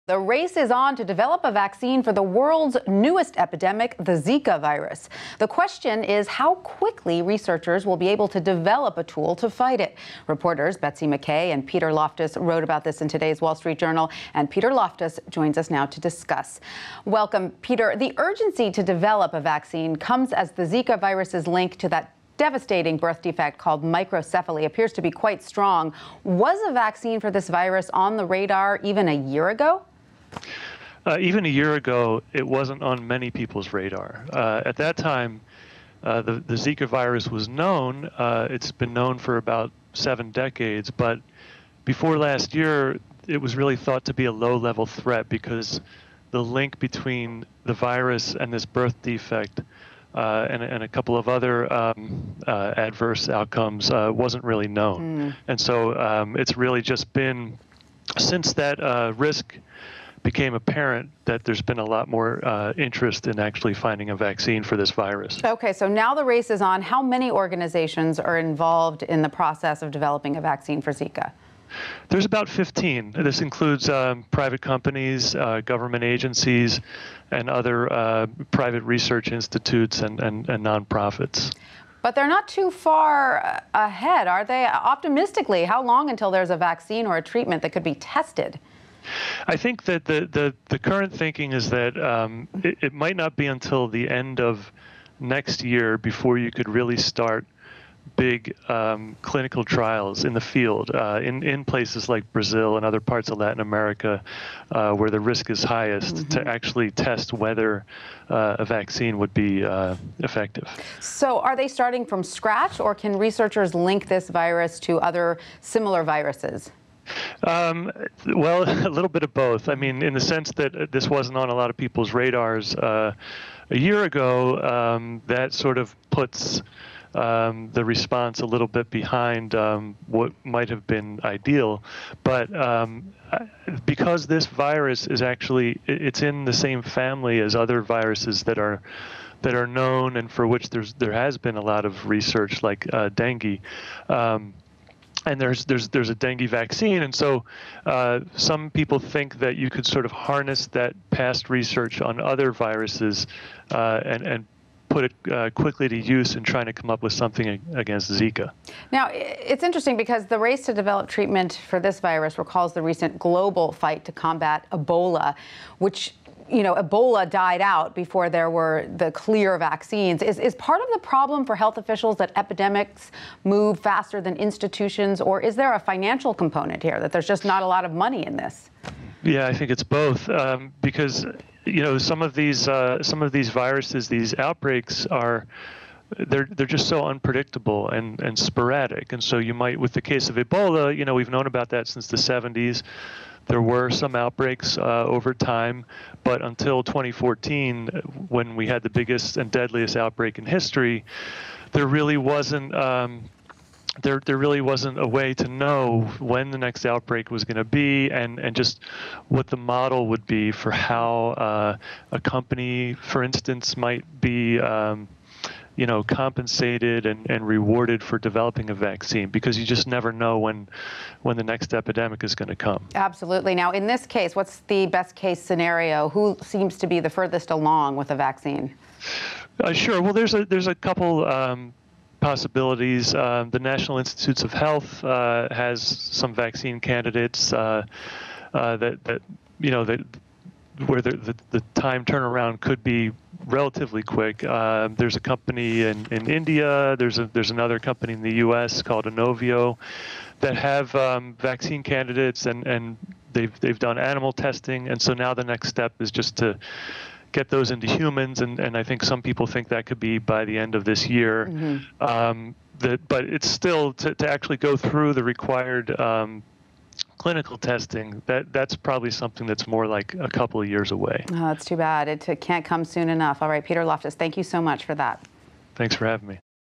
Drugmakers Race to Find Zika Vaccine
Drugmakers are fast-tracking efforts to develop a vaccine to treat the Zika virus, which has been linked to serious birth defects. WSJ's Peter Loftus reports. Photo: Scripps Research Institute